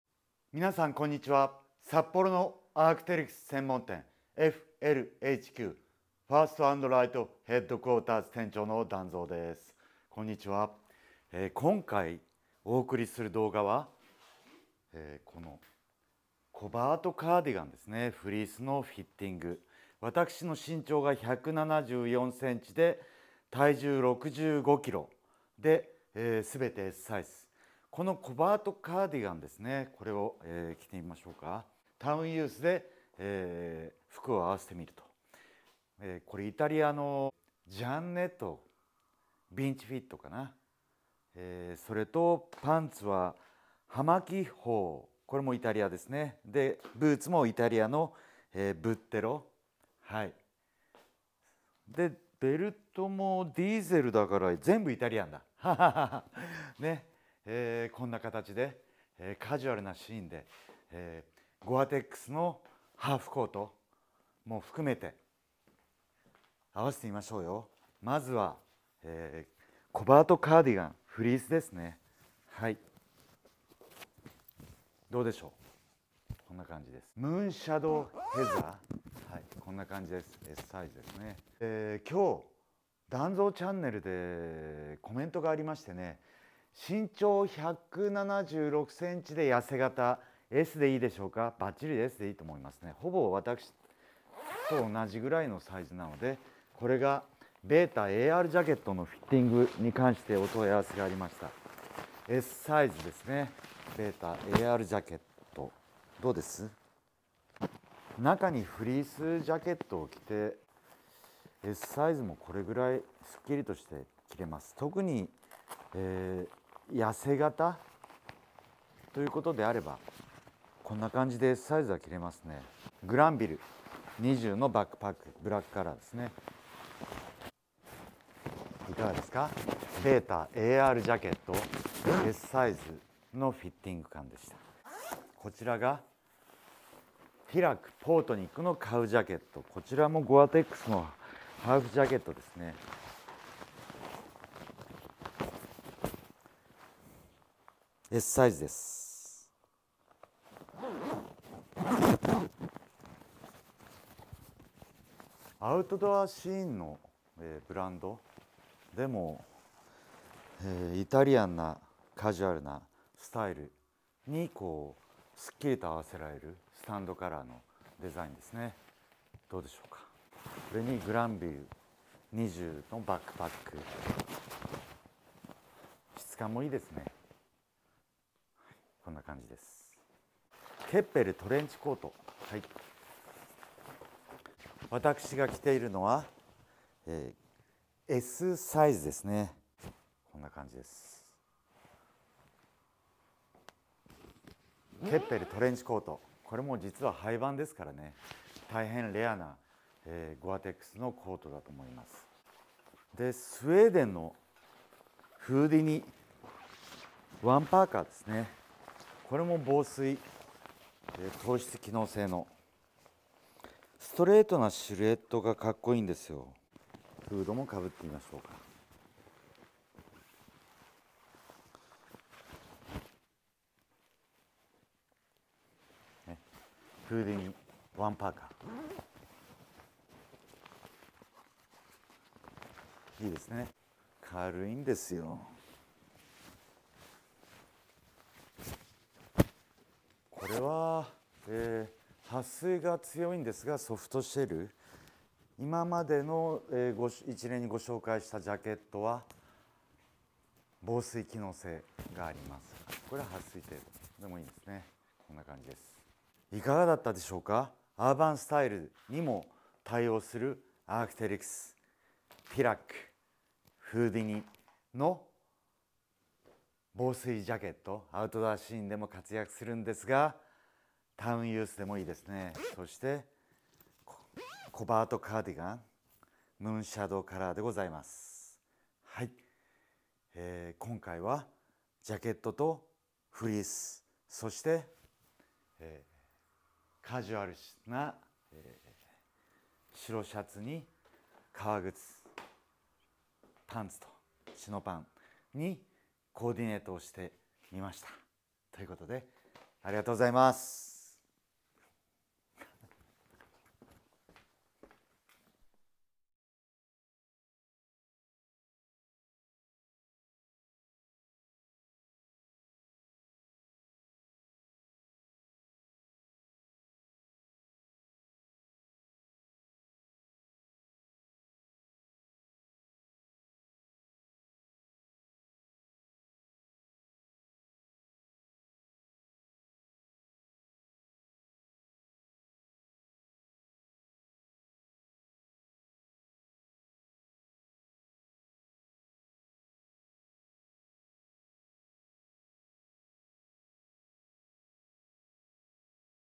【ARC'TERYX】Sサイズのサイズ感について中にフリースを重ね着して説明します。BETA AR JACKET【ファッション】【FLHQ】
今回の動画は、ベータARジャケットに、身長176cm、細身な体型でSサイズで合うか？という視聴者さまから質問がありまして、撮影してみました。街使用のフィッティングに、カジュアルな服、アークテリクスはコバートカーディガンを着用して、ベータARジャケットを着てみました。私は、身長174cm体重65kgでSサイズを着用してます。合わせて、街使用にも対応するアウトドアブランドのジャケットを試着していますので、フィッティングの比較としてください。TILAK(ティラック)のKAW JACKET(カウジャケット)、HOUDINI(フーディニ)のONE PARKA(ワンパーカ)とNOMAD PARKA(ノマドパーカ)、ARC'TERYX(アークテリクス)KEPPEL TRENCHCOAT(ケッペルトレンチコート)を着てみました。
バックパックは、アークテリクスのGRANVILLE20(グランビル20)です。
動画の最後に、私の服を掲載してみましたので、最後までご視聴いただければ幸いです。
シャツは、JIANNETTOのVINCI FIT、パンツはHAMAKI-HO、ベルトはDIESEL、シューズはBUTTERO。全てイタリアのファッションアパレルのブランドです。
こちらも、普段着の参考にしてみて下さい。